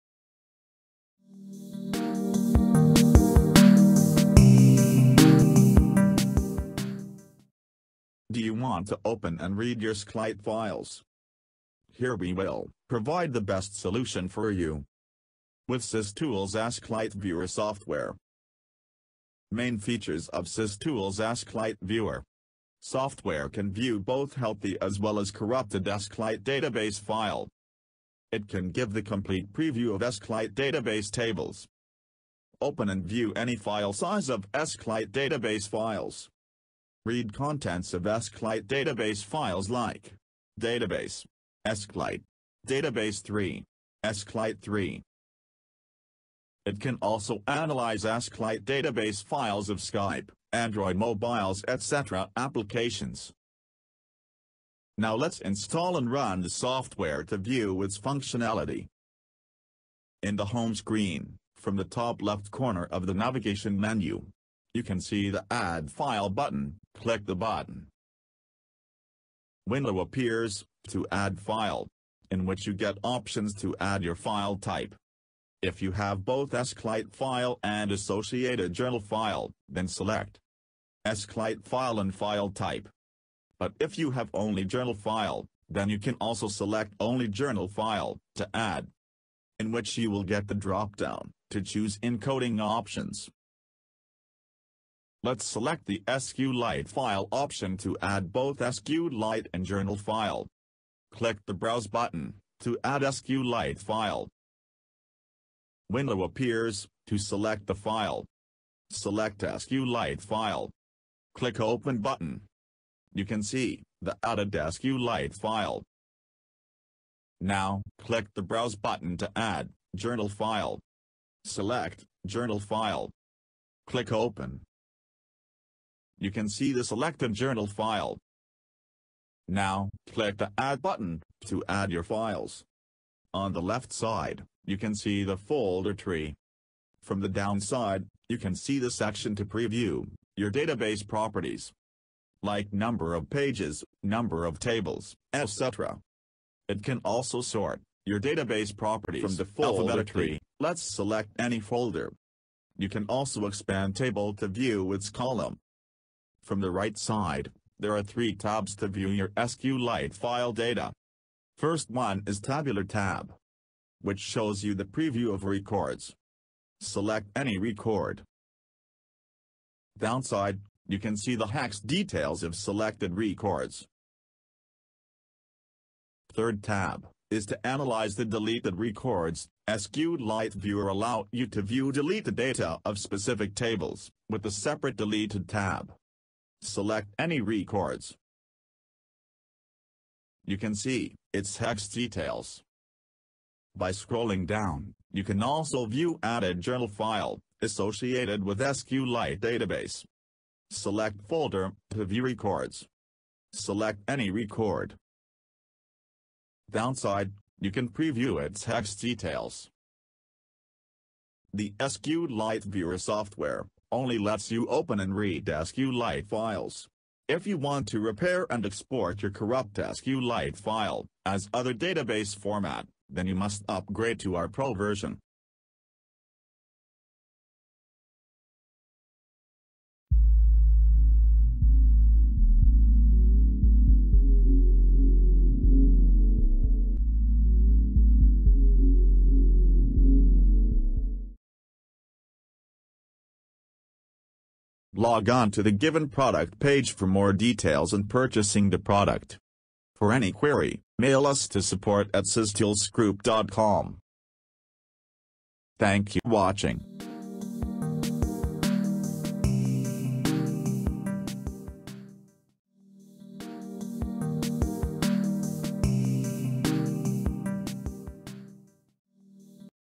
SysTools Sqlite Viewer [Official] - How to View Sqlite Database Using Sqlite Forensics Explorer
SQLite viewer tool to browse, open and extract SQLite database files like *.db, *.db3, *.sqlite, and *.sqlite3. This Windows SQLite database explorer allows you to search, sort and preview Sqlite file data.

Follow the Below Steps to View SQLite DB File:
Step 1: Launch "SysTools Sqlite Viewer"
Step 2: Click on "Add File" button to select an SQLite DB file or journal file
Step 3: Now "browse" the SQLite database files from the destination folder
Step 4: Again Click on "Add button" to View selected database files
Step 5: Software Will "scan and preview" the content of selected files.